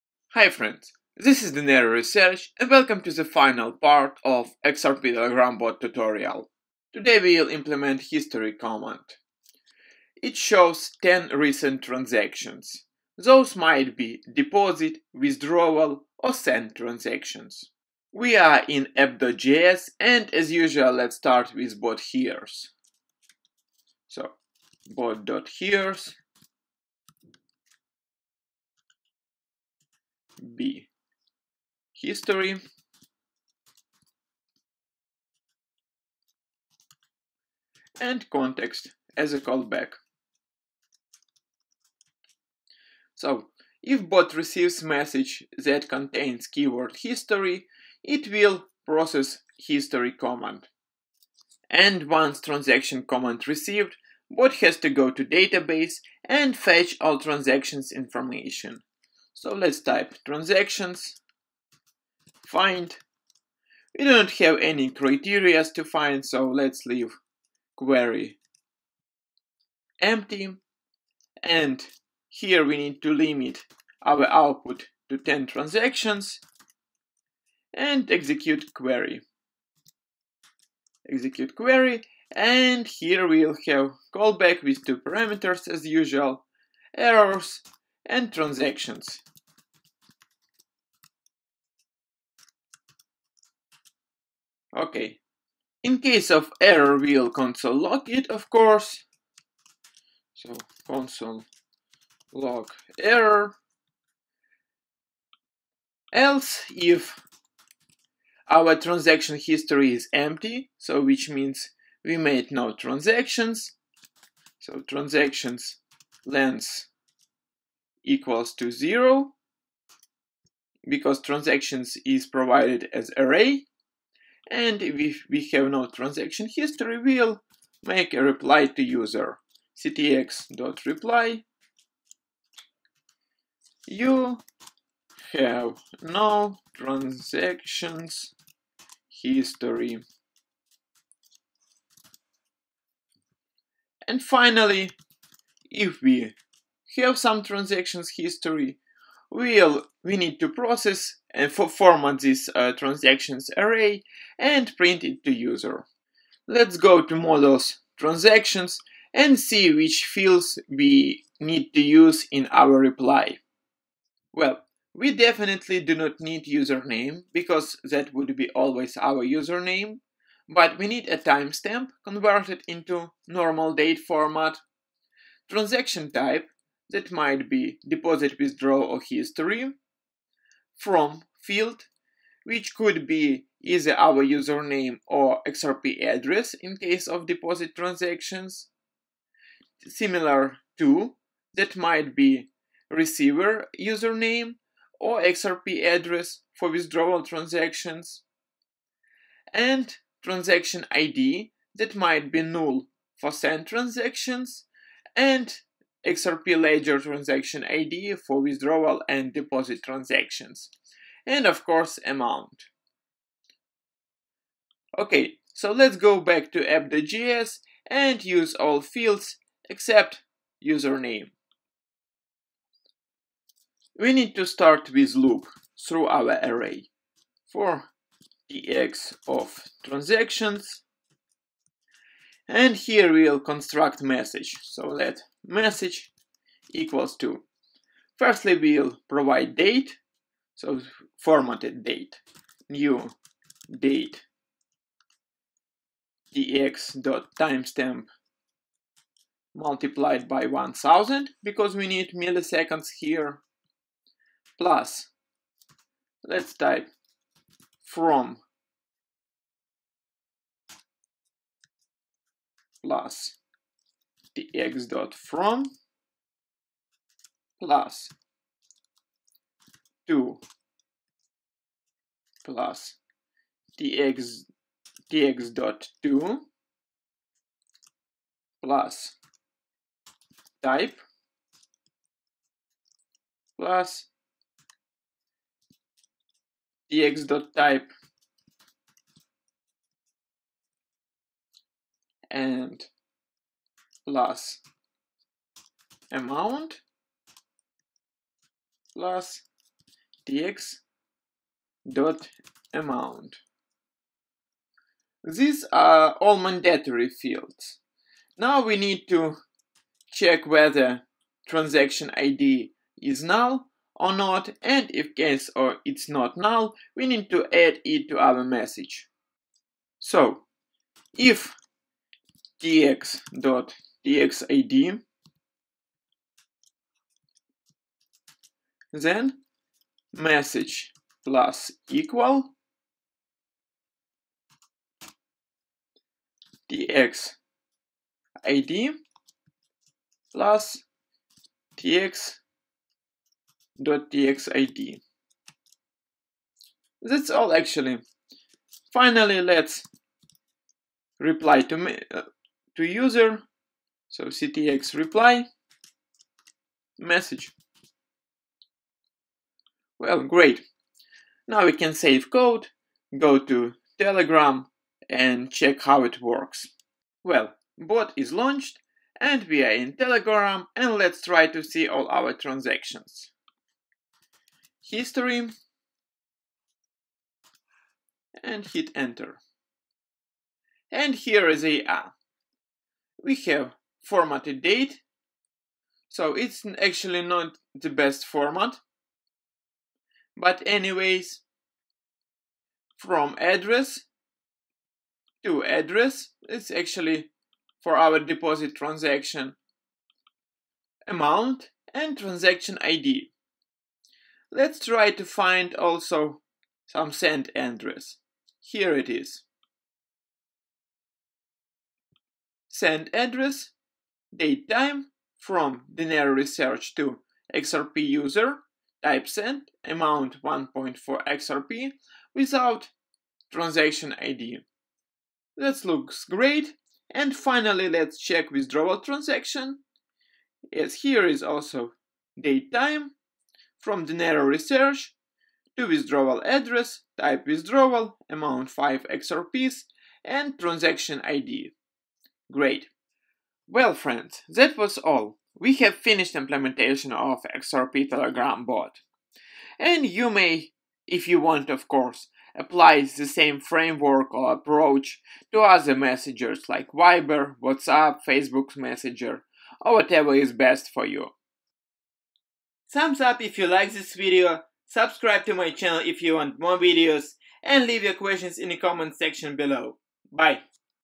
How to build XRP Telegram Bot | Part 13
Denario Research is launching video tutorial series on building XRP tip bot for Telegram. In this tutorials I will show how to use Telegram API, MongoDB and XRP Ledger API.

In this video we finish implementation of XRP Telegram bot. We will add history command.

BTC   1LWLpqLRy9hMiU7EGzNi8bLFyNfegJFU3B
ETH   0xa5b4ee7eb7444688aacd98099223ebd08f0cbde7
LTC    LcsMgPRoV4KiybZRRfM66cy7S5kbbgqwZX
USDT 19ZbKVRTSvAikYXVRHdNicjQga3FiMr5hd